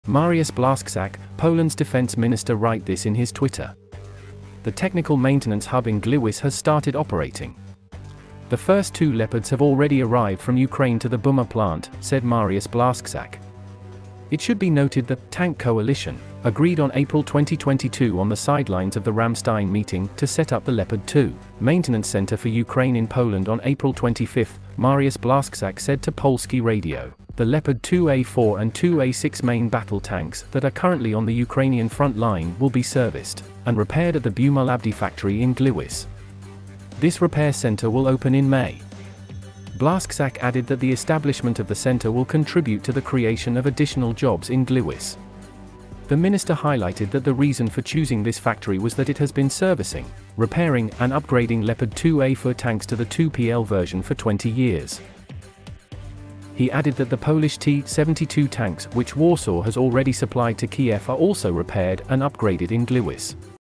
Technical Maintenance Hub Leopard 2 MBT Has Started Operating in Poland
The first two Leopards have already arrived from Ukraine to the Buma plant in Gliwice